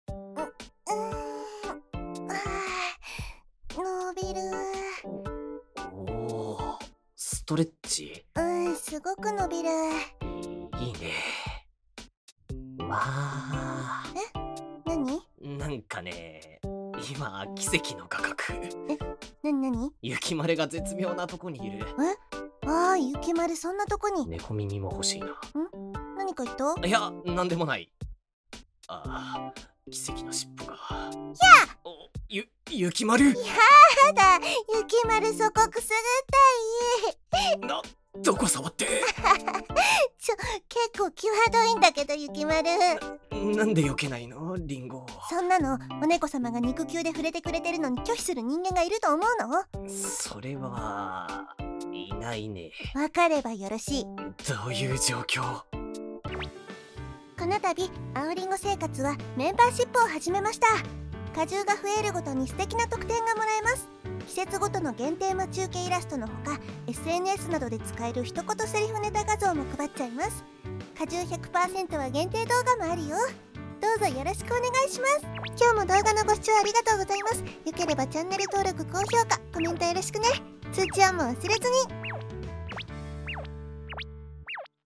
しっぽ付き彼女【アニメ】
絶妙な角度で彼女のお尻からしっぽが……
幼馴染のアオとりんごは、付き合い始めの恋人同士🍏

大学に通うため、両家両親公認で始めたルームシェアから
同棲生活に発展しちゃった二人の日常をお届け♪

火・木・土の18時に投稿予定！
通知ONも忘れずにー！！
🍏出演者
アオ……ちょっと抜けてるところのある男子大学生。りんごのことがナチュラルに好きでとても大切にしている。
りんご……一途がたまに空回りしちゃう女子大学生。アオのことが好きで色仕掛けもしちゃうけど途中で恥ずかしくなるタイプ。

TikTokもやってます！